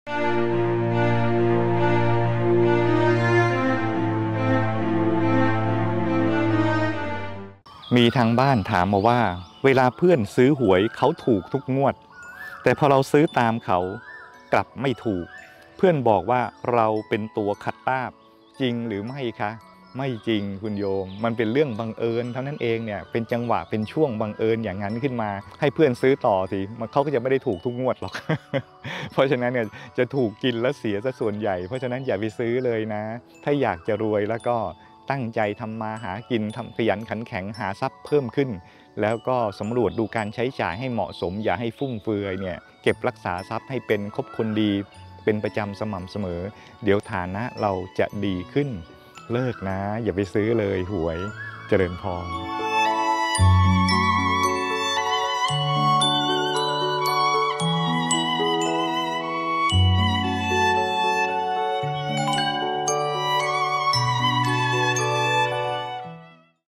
เราเป็นตัวขัดลาภ | EP 2175 | 07-04-67 | ThanavuddhoStoryOfficial
มีทางบ้านถามมา : “เวลาเพื่อนซื้อหวย เขาถูกทุกงวด แต่พอเราซื้อตามเขากลับไม่ถูก เพื่อนบอกว่าเราเป็นตัวขัดลาภ จริงหรือไม่คะ”
พระอาจารย์ตอบ : ไม่จริงคุณโยม มันเป็นเรื่องบังเอิญเท่านั้นเอง เป็นจังหวะ เป็นช่วงบังเอิญอย่างนั้นขึ้นมา ให้เพื่อนซื้อต่อสิ เขาก็ยังไม่ได้ถูกทุกงวดหรอก เพราะฉะนั้น จะถูกกินและเสียส่วนใหญ่ เพราะ ฉะนั้น อย่าไปซื้อเลย ถ้าอยากจะรวย ตั้งใจทำมาหากิน ขยันขันแข็งหาทรัพย์เพิ่มขึ้น แล้วก็สำรวจดูการใช้จ่ายให้เหมาะสม อย่าให้ฟุ่มเฟือย เก็บรักษาทรัพย์ให้เป็น คบคนดีเป็นประจำสม่ำเสมอ เดี๋ยวฐานะเราจะดีขึ้น เลิกนะ อย่าไปซื้อเลย หวย เจริญพร.

สมัครสมาชิก สนับสนุนผลิตสื่อธรรมะ: